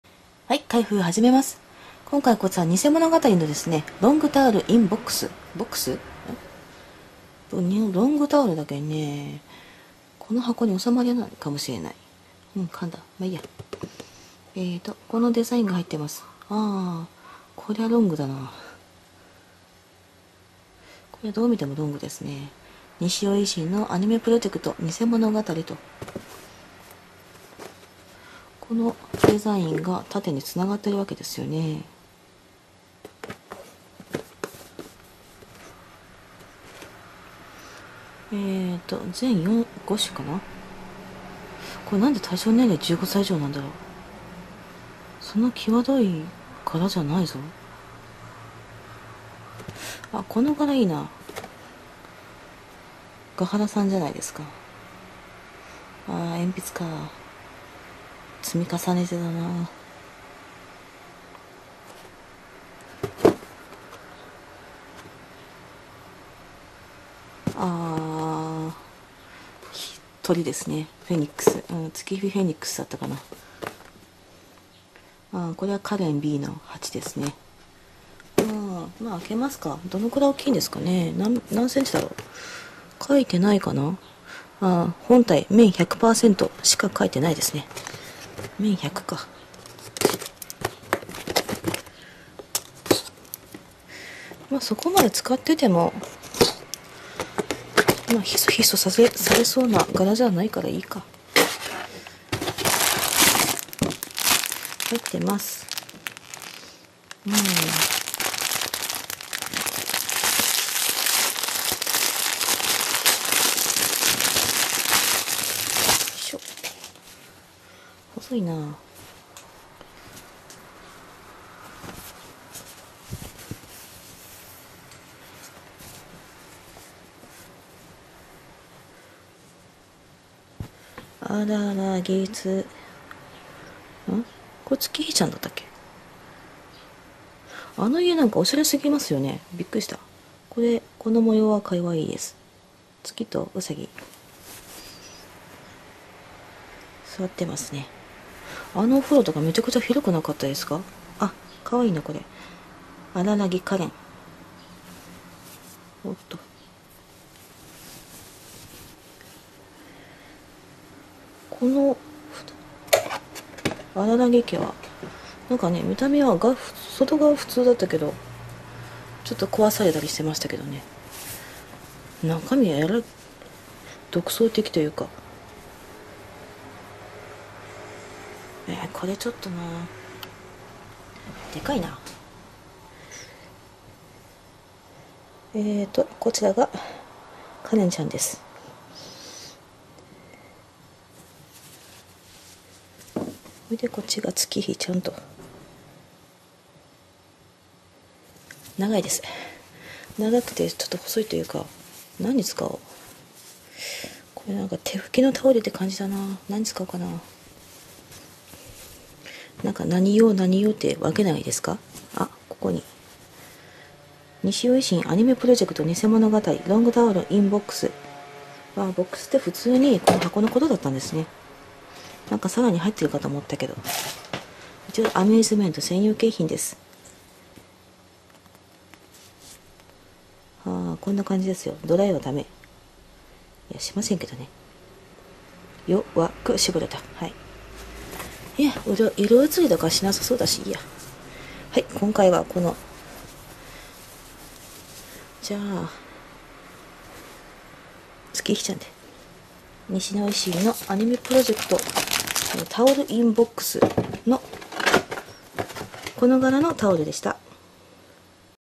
UFOキャッチャー開封：偽物語ロングタオルINボックス 月火と火憐
箱に入ったロングタオルを開封しました。
阿良々木家の内装っぽいデザインです。
阿良々木家の内装いいですよね。すごい広いし。
玄関壊された後どうしたんでしょうか。